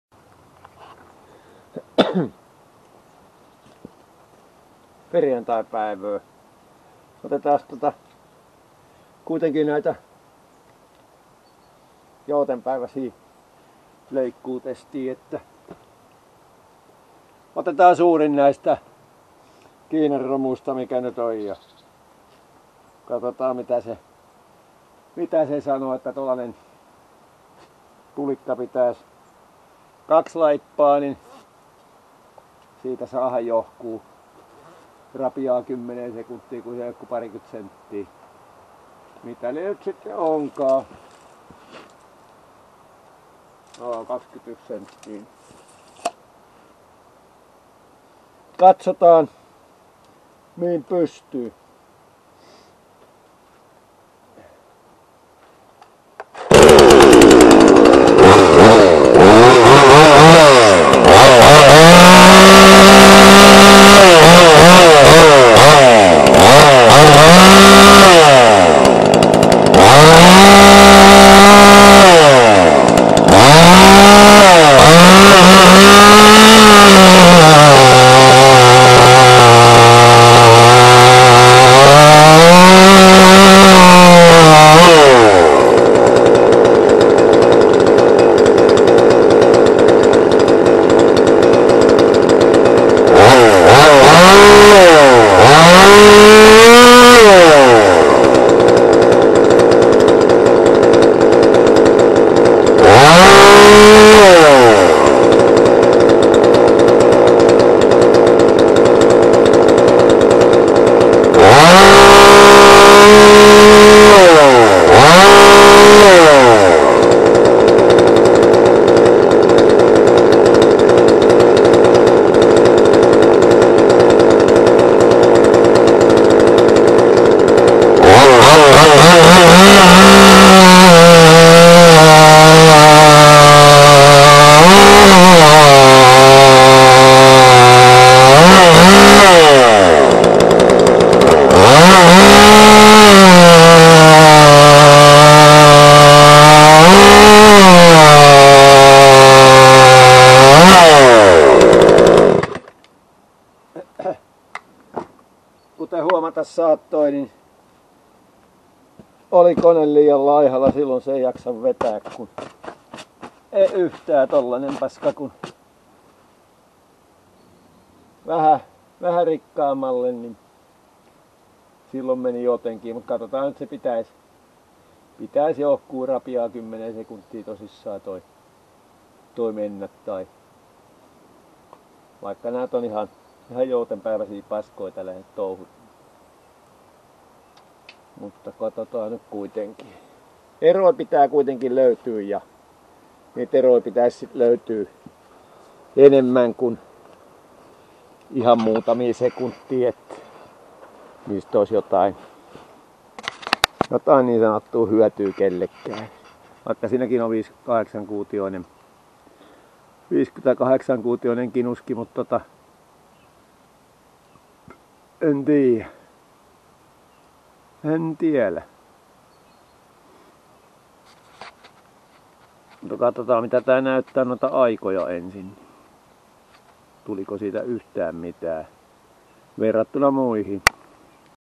Benson 58cc
Benson 58cc kinuski 20" laipalla videolta katottuna sain aika ok leikkuunopeuden. 52cc sahat hävisi sen 4-5 sek